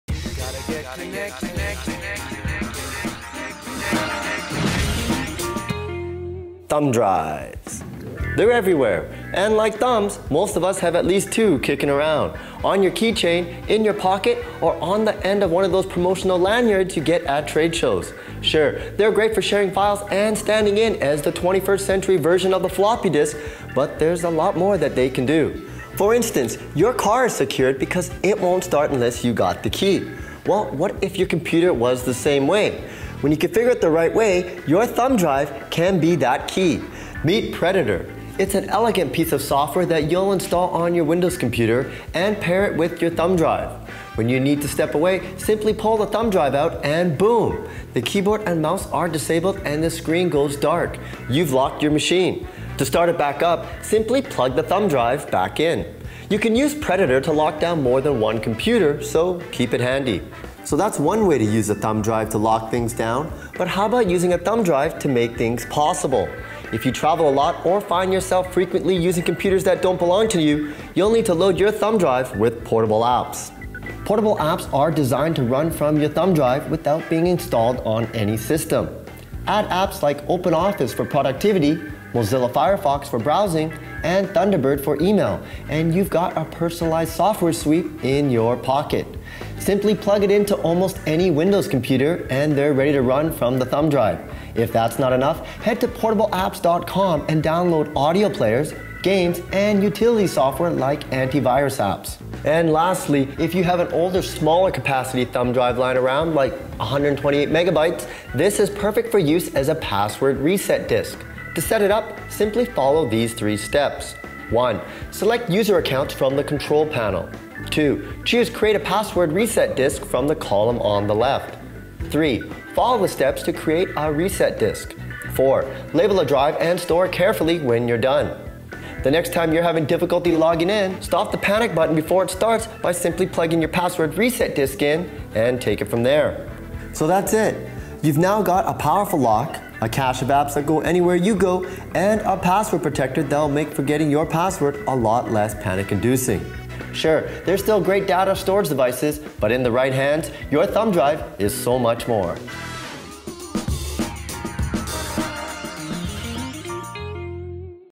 USB Thumbdrives For Security and Portable Apps and As A Password Reset Disk
Did you know that you can use your USB thumbdrive for all sorts of cool things? We show you how you can use your USB key to lock down a computer for security, to access portable apps whereever you go and to create a password reset disk. You probably already have a USB thumbdrive with you most places you go, we show you how to get the most out of it.